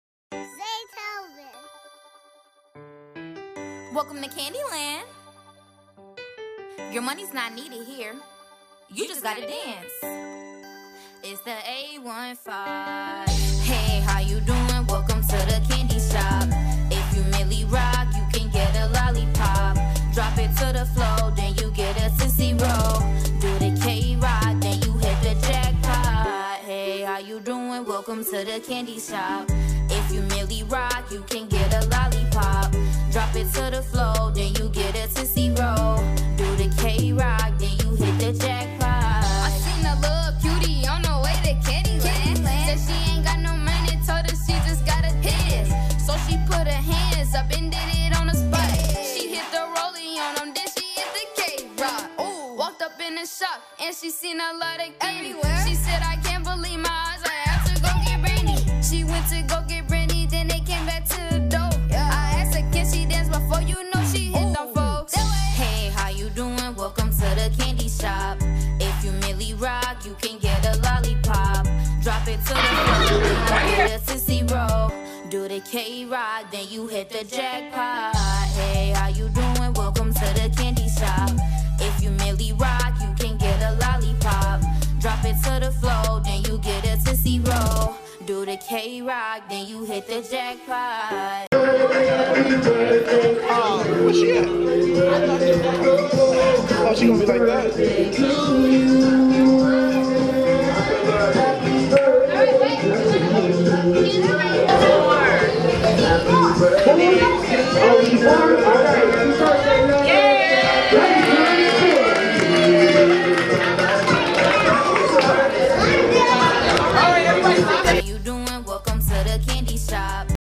London's 4th Birthday Party @1ofakindgear | Candy Land Theme | The815
Here's an inside look at our very 1st face painting event: London's 4th Birthday Party! Book us now for your next @1ofakindgear [1 Of A Kind™ face painting]

-Location: Cardinal Shehan Center - Bridgeport, CT (3-17-18)